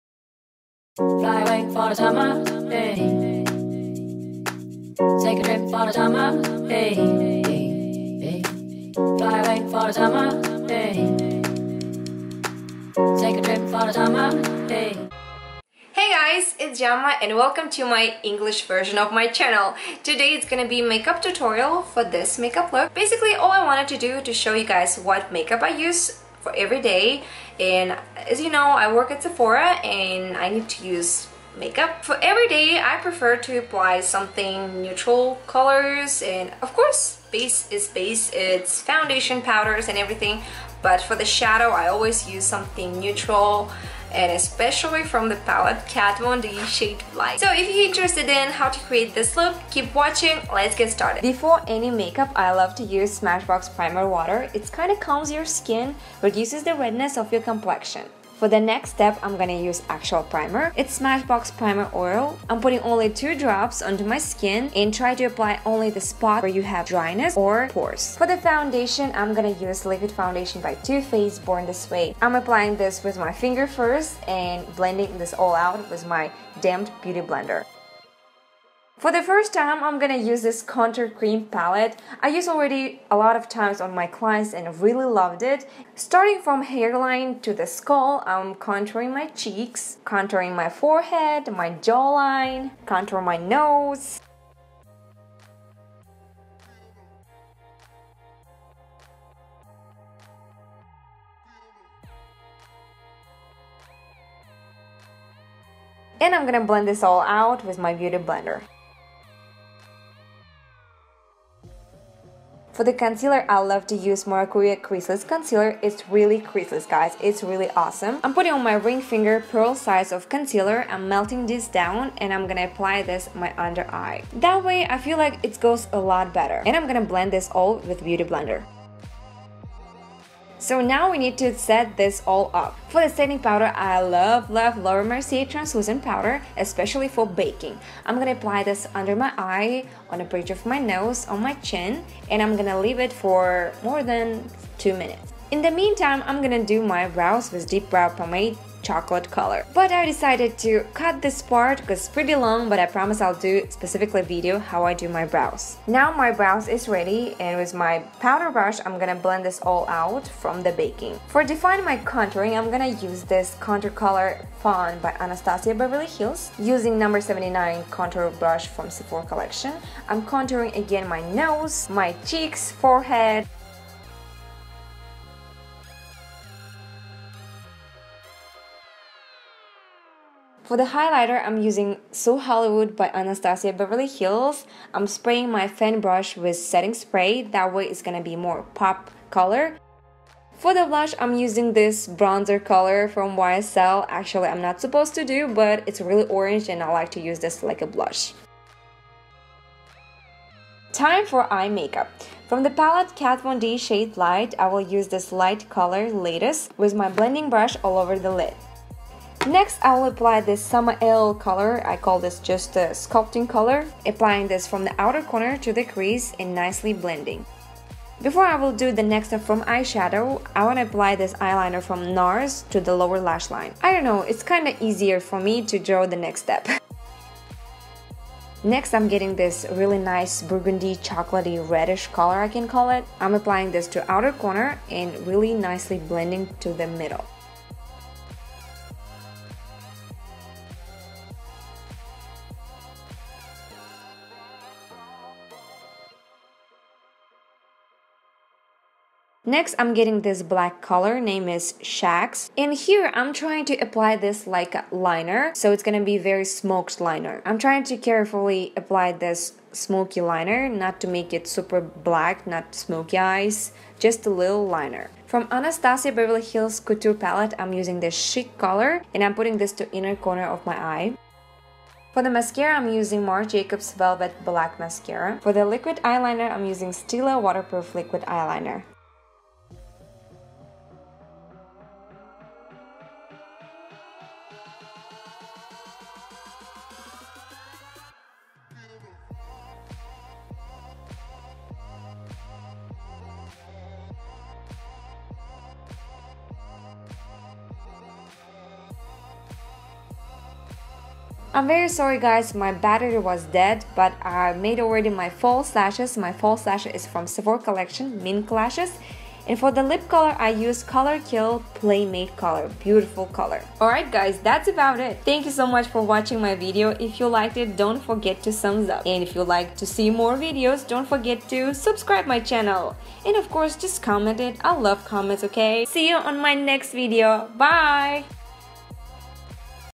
MY EVERY DAY MAKEUP ROUTINE Jamkin Donuts {English}
in this video I'm showing you guys my every day makeup routine, for some of you, it might quite a lot of makeup, but reality is I work at Sephora, and I work on full time, basically 90% of my life i spend at Sephora haha! thats why I need to wear ton of makeup, and I love it! its passion! Hope u enjoy a video!! mwah!

XO,XO Jamkin Donuts

S N A P C H A T : Jamkin_Donuts